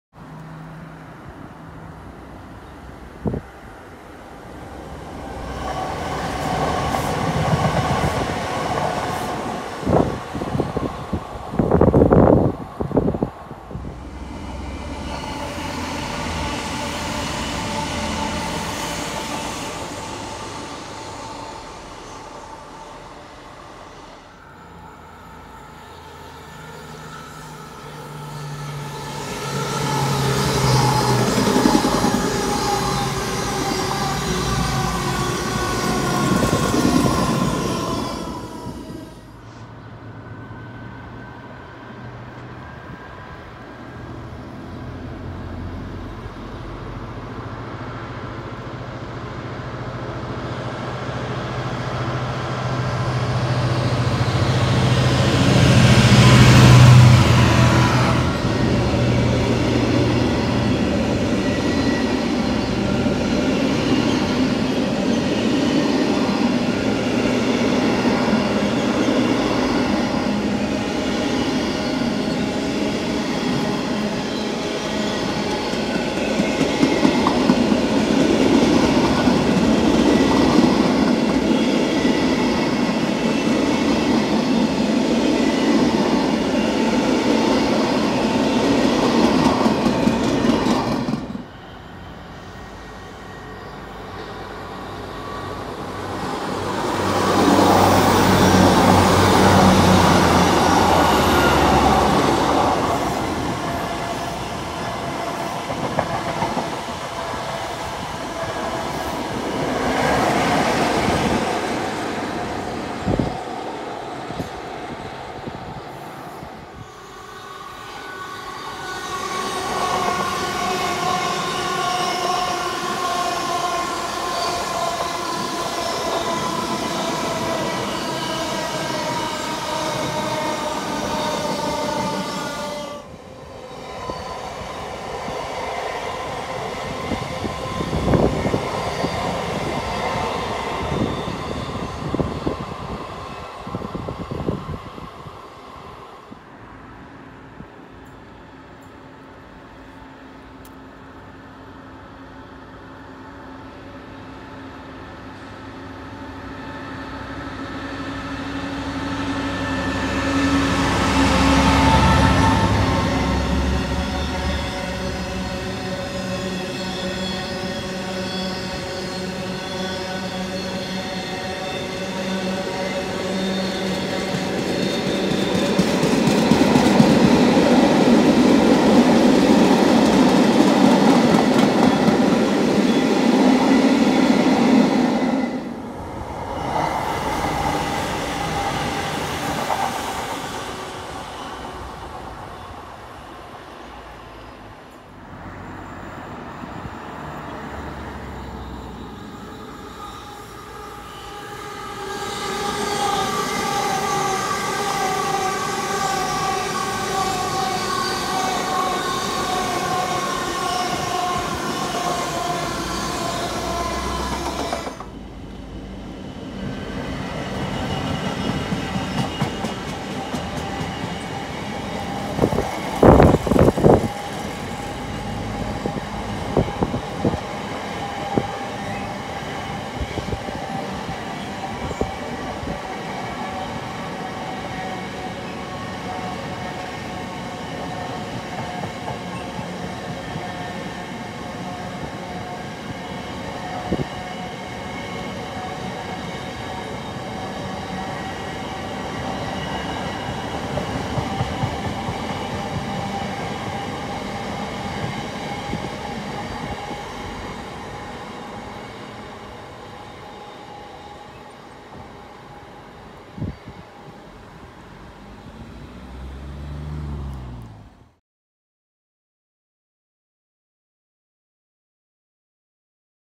HST & IEP Diversions - Patchway
This week saw the continuation of services from London to Wales diverting through Filton Abbey Wood due to the electrification of the line between Swindon and Bristol Parkway. On this occasion there was a HST (which is becoming far rarer on South Wales services), IEP Class 800s and also some freight services, with Coal, Metals and Cargowaggon trains all hauled by Class 66s.